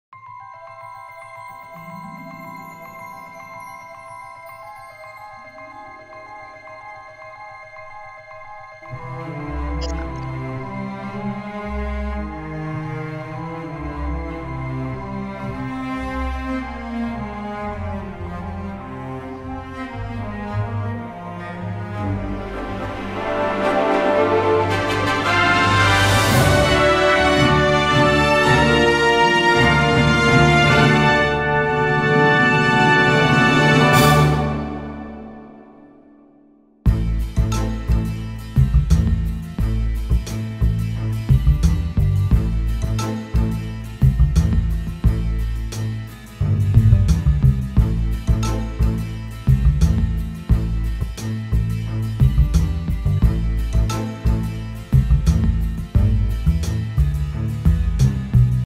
Intro for online event - Janssen Oncology Mission AMY
Intro made for a Janssen online event with a Sci-Fi theme

We are a video production company specialized in projects like this, for corporate lives.